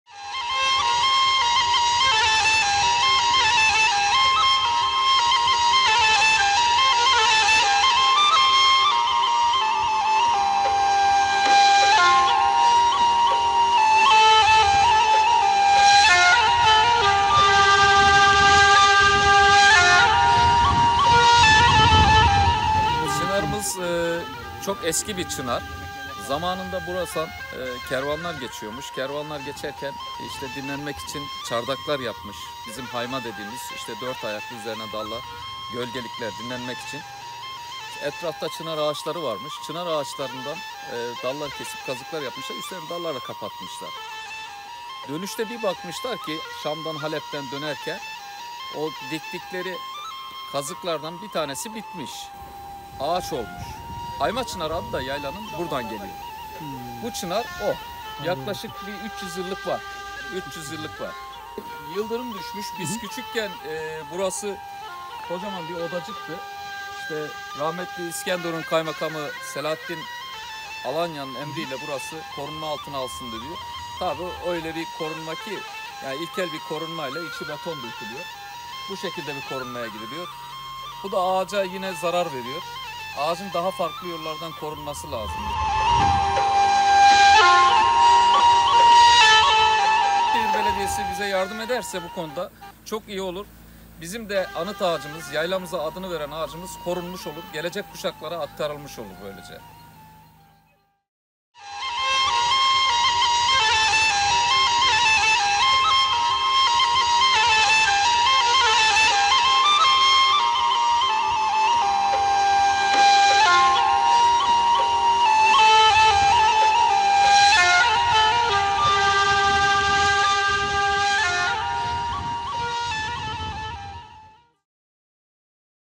300 yıllık anıt çınara HBB’den restorasyon bekleniyor...
Hatay'ın İskenderun ilçesine bağlı Haymaçınar yaylasına adını veren yaklaşık 300 yıllık anıt çınar için yaylacılar Büyükşehir belediyesinden restorasyon yapmasını istiyor.